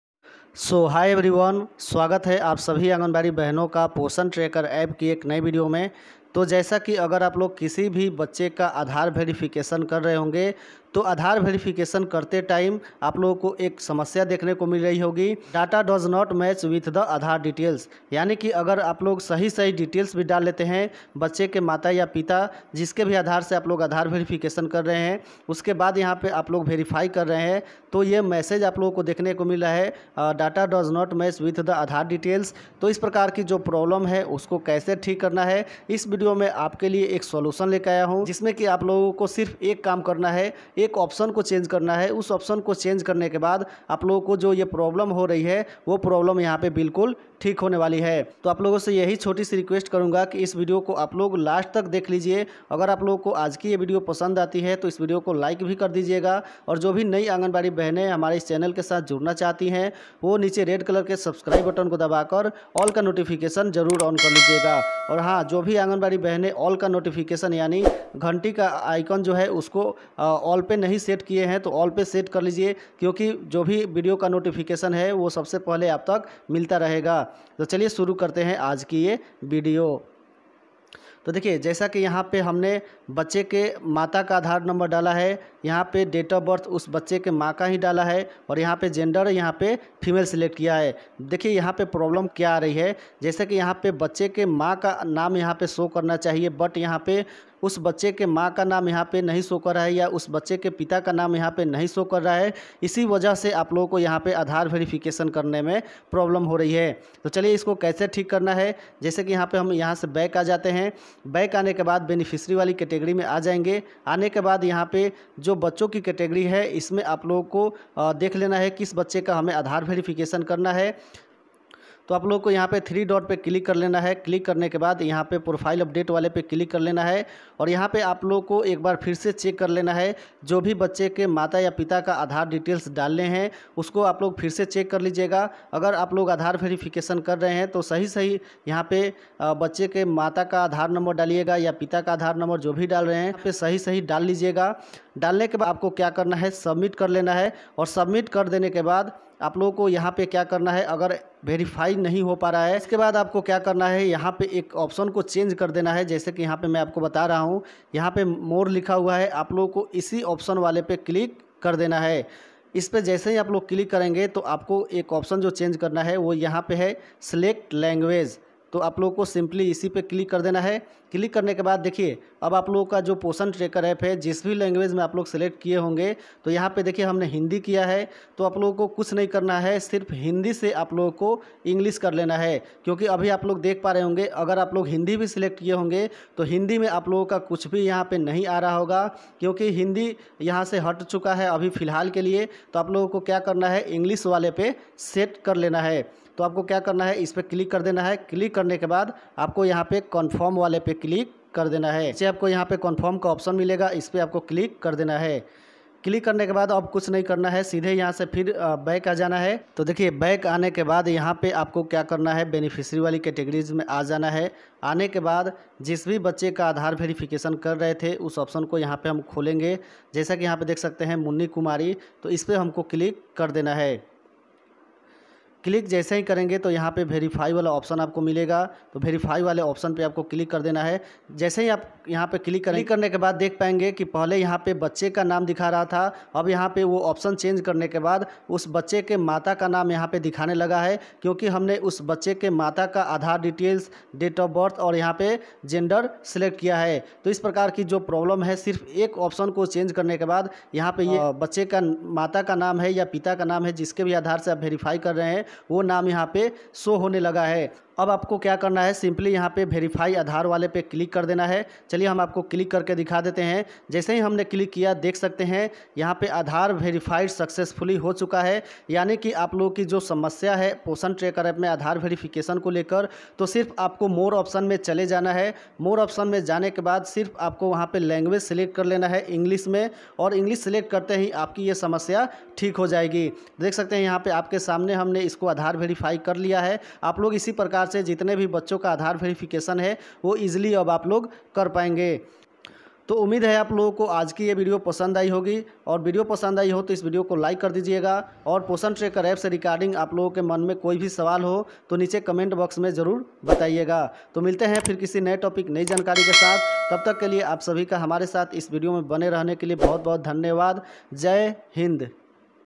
आधार वेरीफाई 2 मिनट में!poshan tracker verify aadhar! माता पिता का नाम कैसे show करें!
पोषण ट्रैकर में 0-6 से 6-3 वर्ष में और 6-3 से 3-6 वर्ष  के कैटेगरी में बच्चों का नाम कैसे बदले! आसानी से!👇

लाइक कीजिए 👍
कमेंट कीजिए...